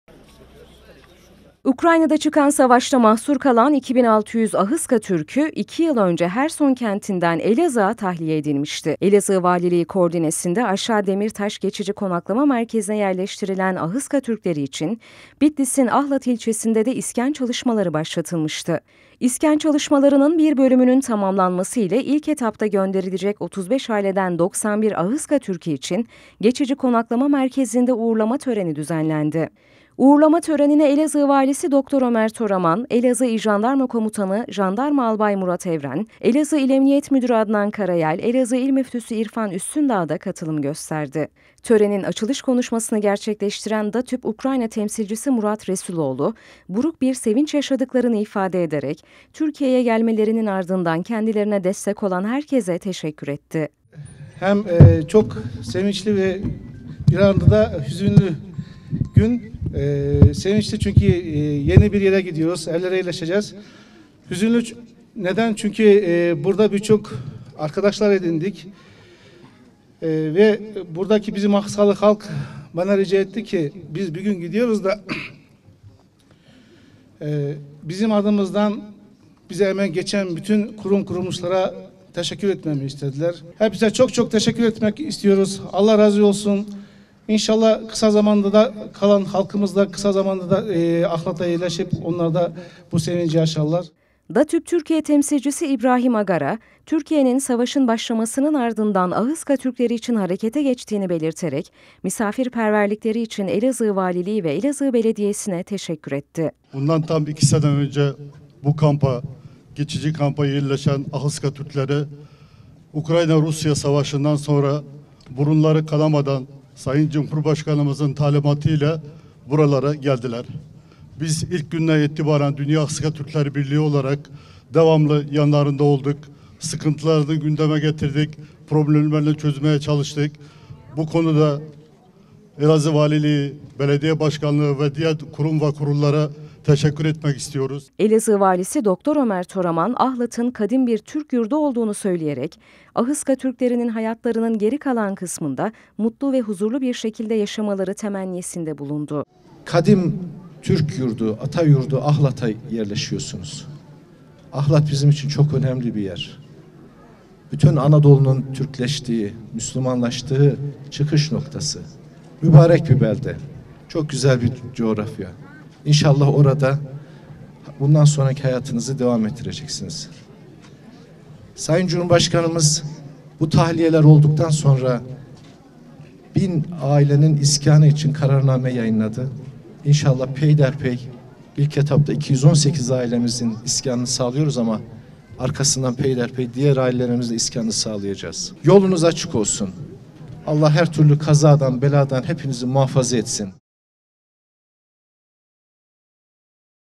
AHISKA TÜRKLERİ YENİ YUVALARINA UĞURLANDI
UKRAYNA’DAKİ SAVAŞ BÖLGESİNDEN TAHLİYE EDİLEREK ELAZIĞ’DA GEÇİCİ KONAKLAMA MERKEZİNE YERLEŞTİRİLEN AHISKA TÜRKLERİ, İSKAN İÇİN BİTLİS’İN AHLAT İLÇESİNE UĞURLANDI